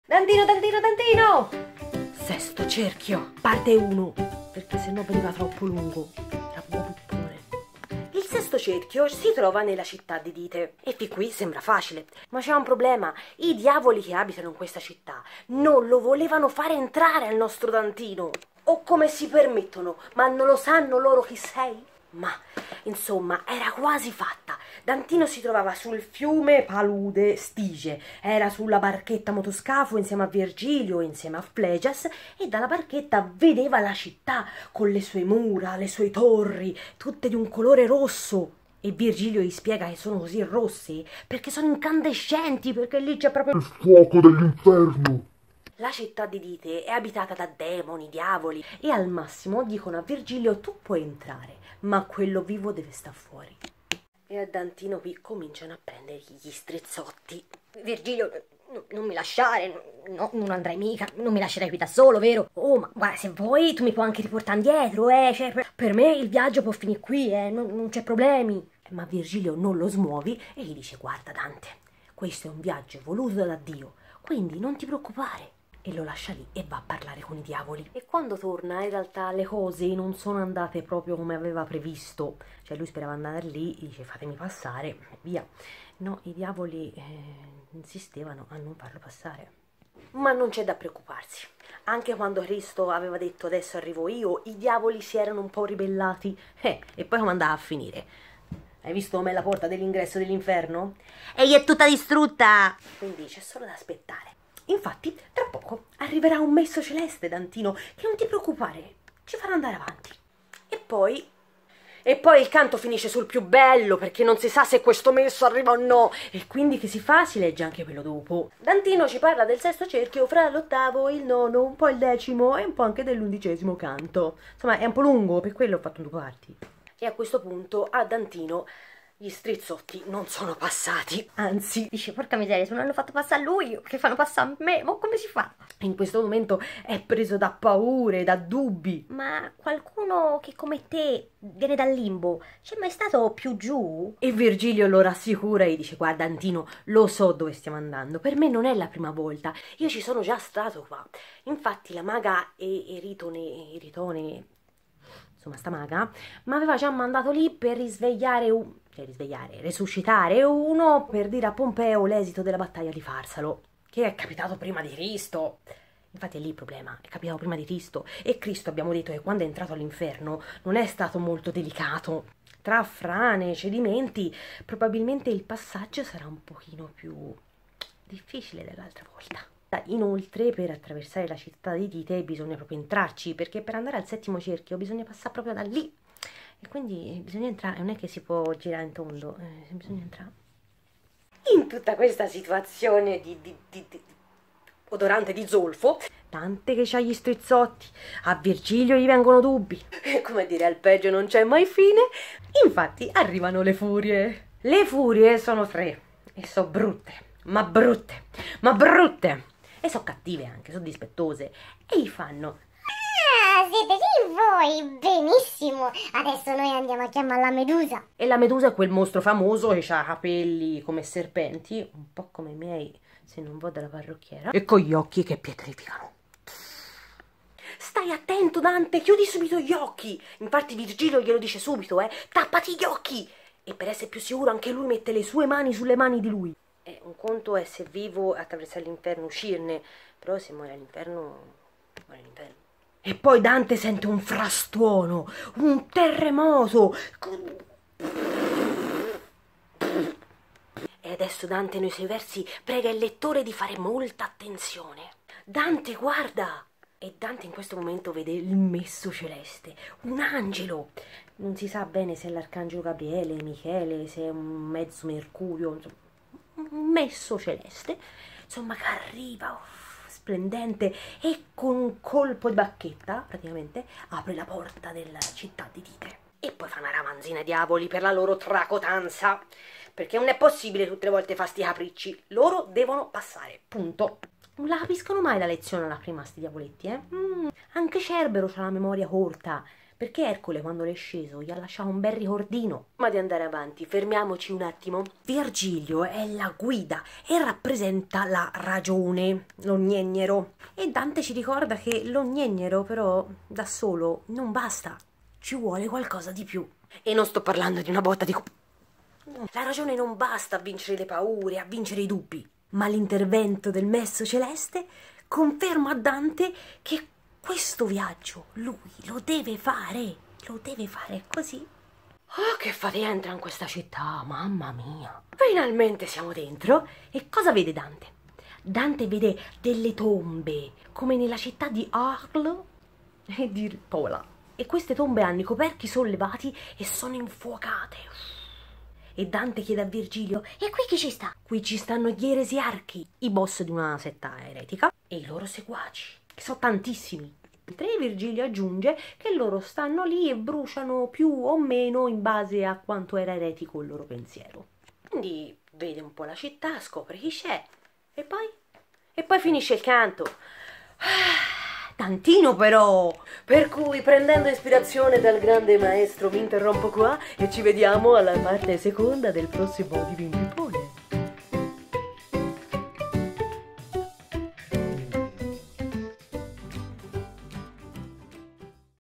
DIVIN PIPPONE: VI Cerchio (parte 1). Inferno. Divina Commedia
Dantinoooooo!
Divin Pippone: VI CERCHIO dell'Inferno!

Oh com'è lungo! L'ho diviso in due... altrimenti sarebbe stato proprio un vero pippone...

Dante stavolta ci dice che a volte nella vita solo la ragione non basta, ci vuole qualcosa di più e non intende una botta di c... Dantinoooo!

Buona visione e riflessione!

Un abbraccio
L'Anna